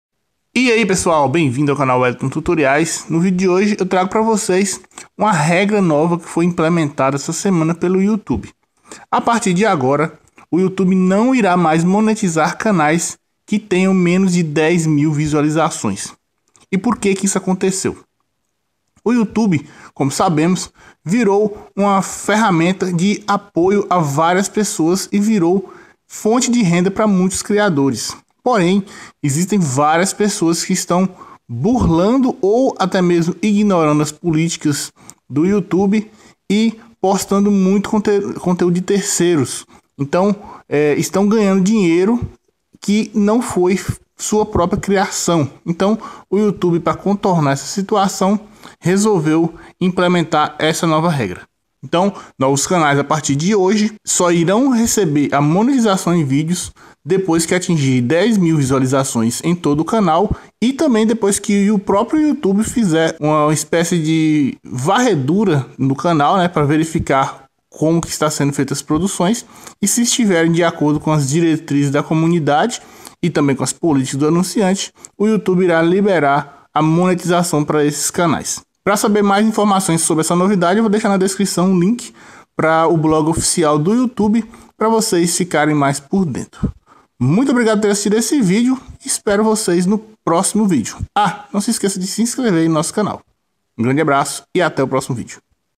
Novas regras de monetização no Youtube - Só com 10 mil views!
Youtube muda as regras e agora só canais com mais 10 mil visualizações terão os vídeos monetizados.

→ Gostou do vídeo? Clique em gostei e deixe seu comentário.

Welington Tutoriais é um canal de tecnologia no Youtube.